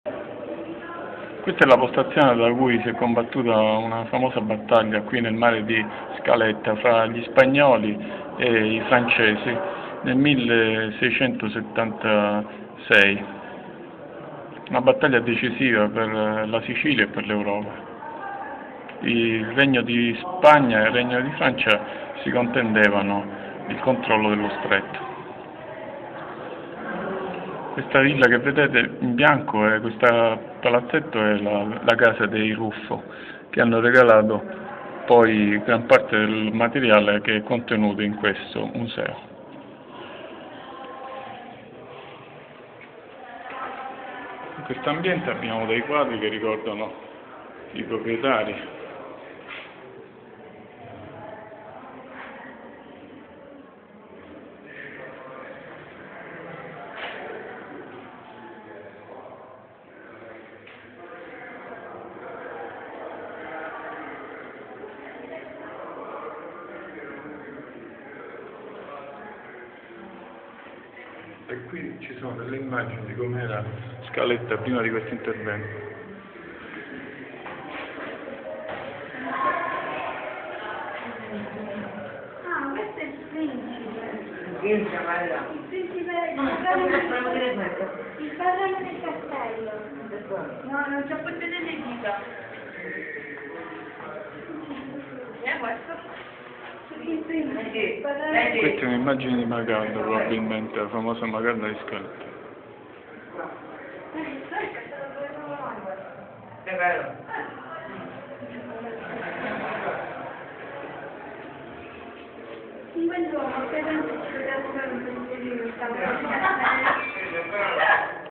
SCALETTA ZANCLEA MESSINA SICILIA SICILY CASTELLO MACALDA VESPRO SICILIANO 1282
CASTELLO SCALETTA MUSEO ZANCLEA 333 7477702
Museo del Castello di Scaletta Zanclea. Nelle sale del rinnovato castello si possono vedere documenti, armi e cimeli della storia della Sicilia, dal 1282 al 1860. Molte donazioni della famiglia Ruffo di Scaletta e acquisizioni molteplici provenienti anche da prestiti di cittadini e istituti culturali. Una località spettacolare, val bene una salita al Castello il magnifico panorama e l'aria pulita del paesino ormai quasi deserto.  Un luogo di meditazione e di storia. Macalda di scaletta fu una dele figure della Guerra del Vespro, moglie di Alaimo da Lentini, comandante della piazza di Messina, nella guerra contro l'esercito angioino derante l'asedio di Messina, dal maggio al settembre 1282.  La Guerra del Vespro sconvolse l'Europa , un Popolo cristiano aveva vinto contro un papa di roma ed il suo sceriffo, Carlo d'Angiò. Museum of the Castle of Zanclea Ladder. In it knows them of the renewed castle can be seen documents, you arm and cimeli with the history of the Sicily, from 1282 to 1860. Many coming donations of the family Ruffo di Scaletta and multiple acquisitions also from loans of citizens and cultural institutes. A spectacular locality, val well a climb to the Castle the magnificent panorama and l' clean air of the by now nearly desert small village. A history and meditation place. Macalda of ladder was dele figures of the War of the Vespro, wife of Alaimo from Lentini, commander of the public square of Messina, in the war against l' derante army angioino l' asedio of Messina, from the May to september 1282. The War of the Vespro upset l' Europe, Christian Popolo had gained against a Pope of Rome and its sceriffo, Carl d' Angiò.